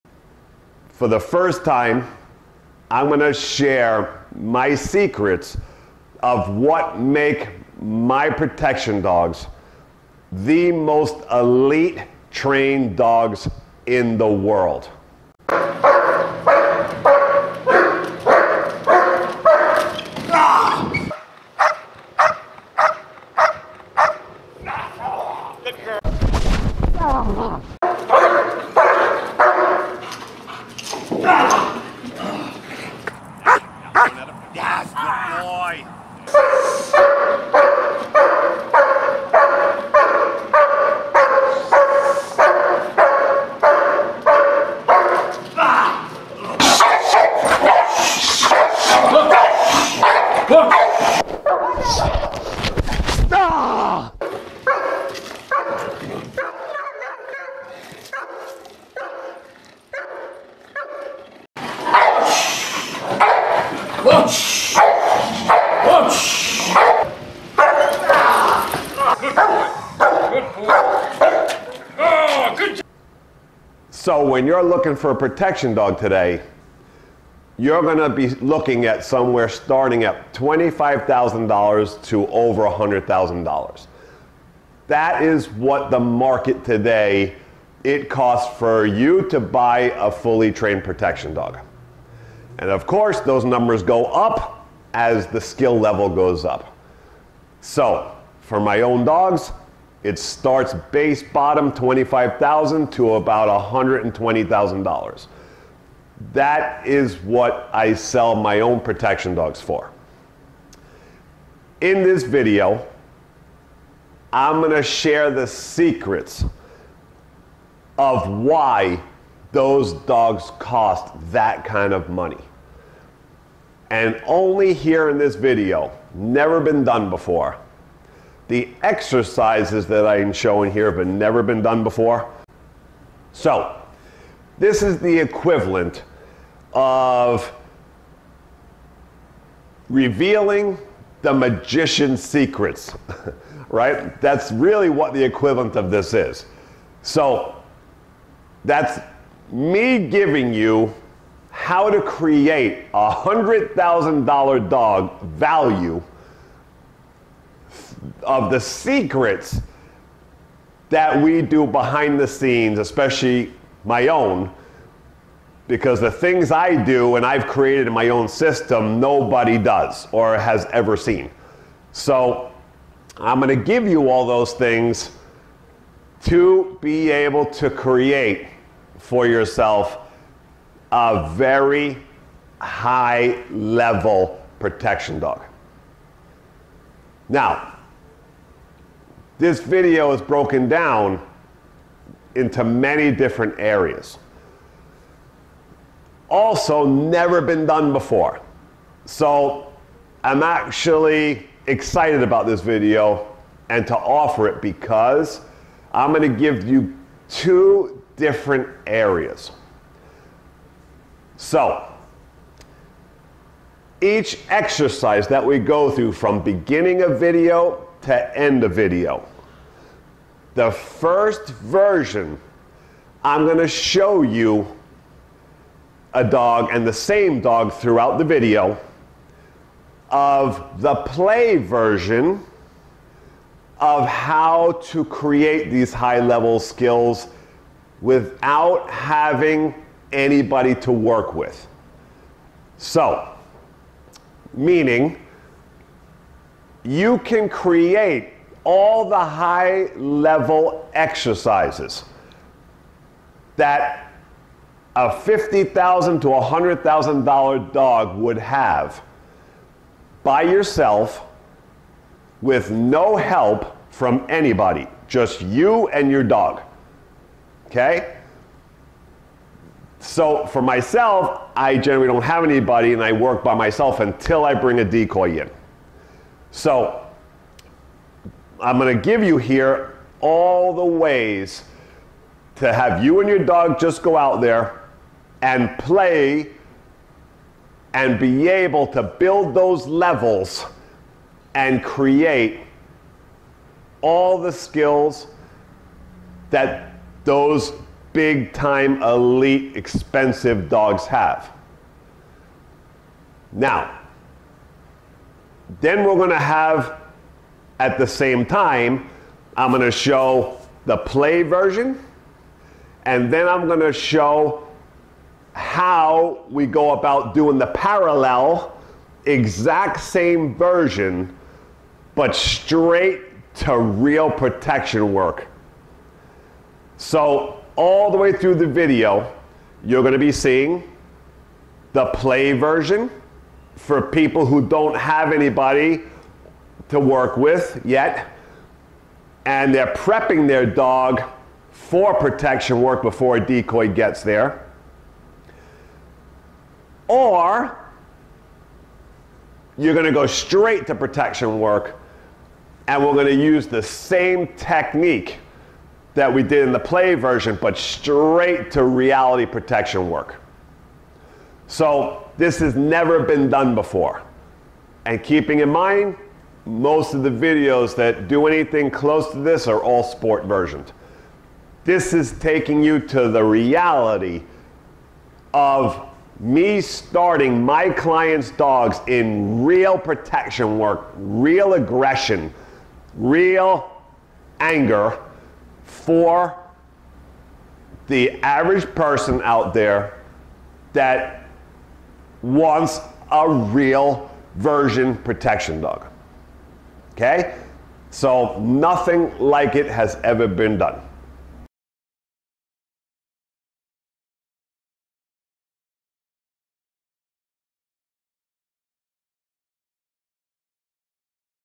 SECRETS of training Elite Protection Dogs Instructional video
MASTER CLASS do it yourself protection dog series!
Richard reveals his secrets for the first time ever on how he trains the best protection dogs on the planet!
In this instructional video Richard takes you step by step teaching you the exact exercises that create 25,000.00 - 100,000.00 dollar protection dogs.